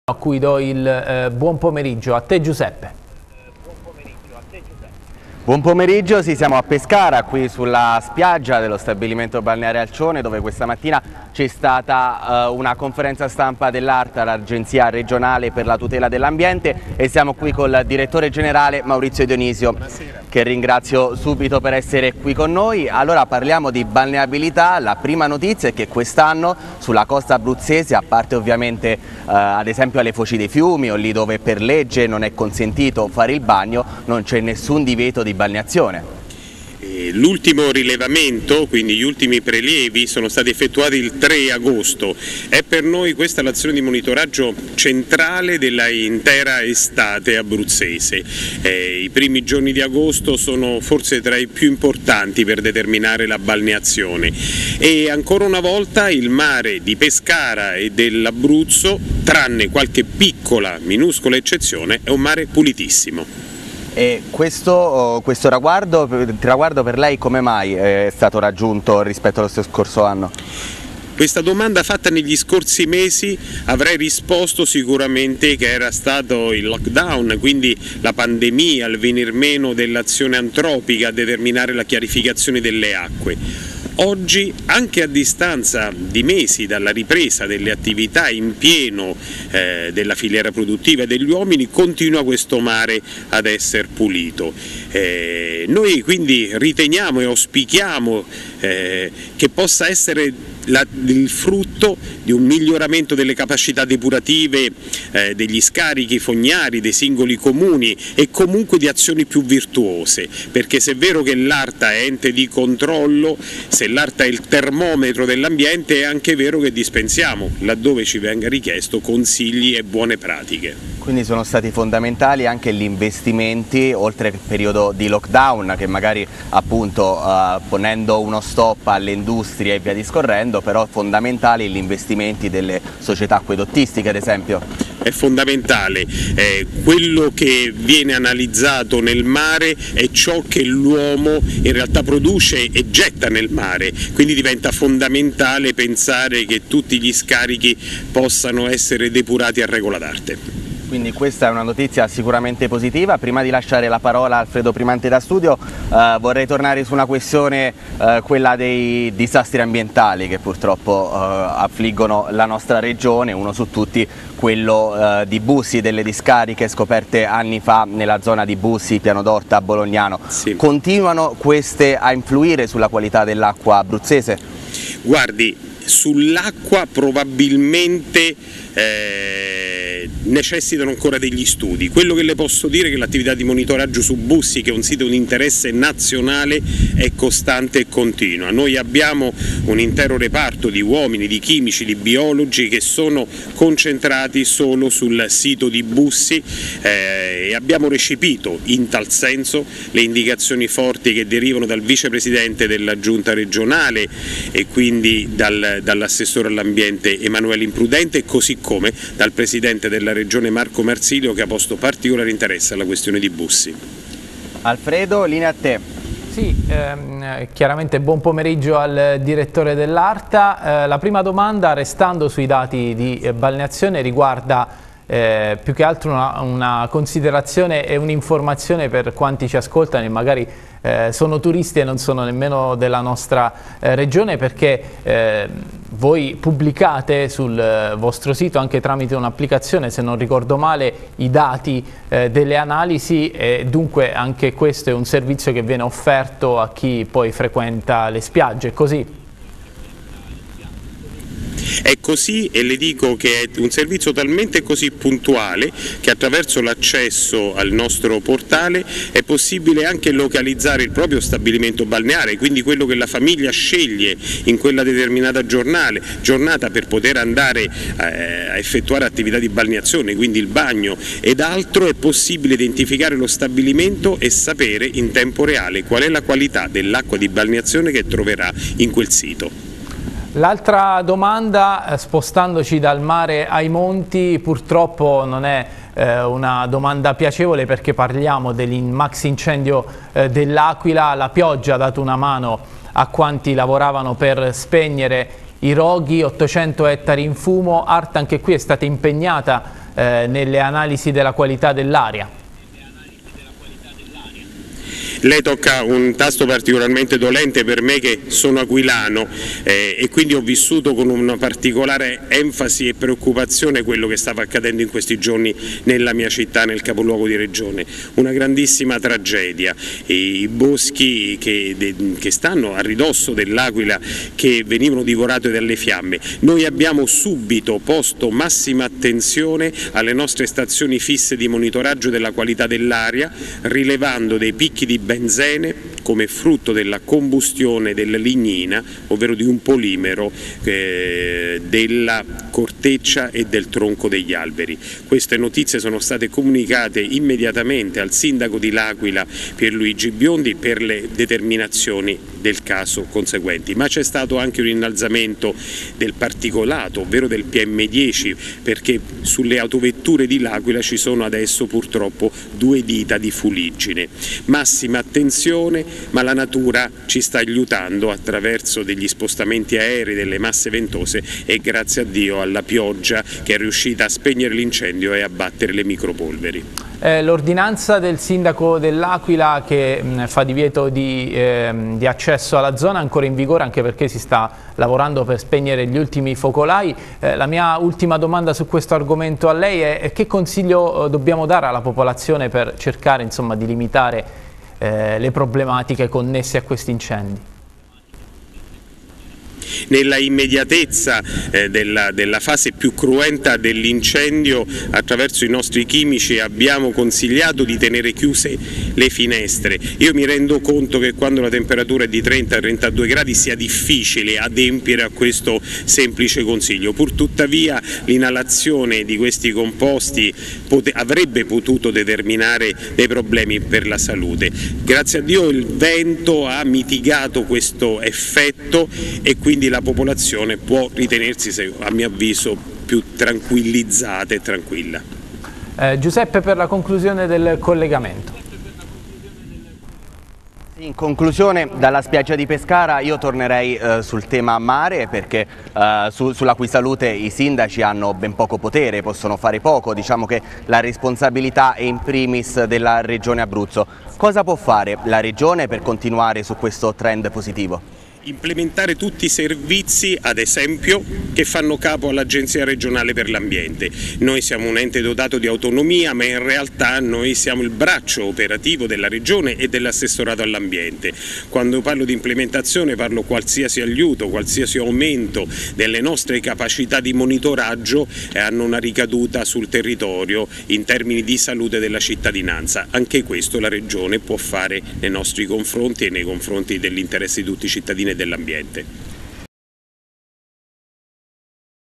Arta. Dati positivi per il mare abruzzese
Il mare della costa abruzzese è pulito e balneabile. E' il risultato della analisi svolte dall'Arta dopo gli ultimi prelievi effettuati due giorni fa. Lo conferma, nel corso del collegamento al TgSei, il direttore generale dell'ente Maurizio Dionisio con cui abbiamo parlato anche dell'emergenza incendi a L'Aquila